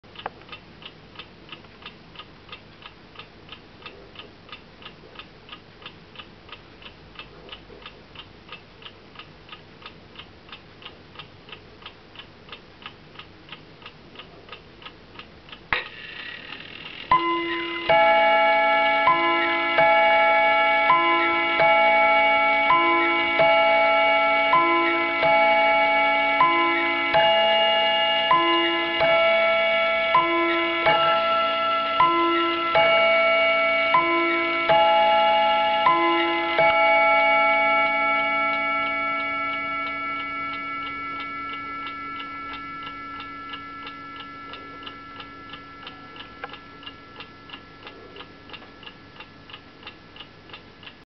Small Hermle movement
A small Hermle movement with floating balance and ting tang chimes, new mainsprings and barrels fitted.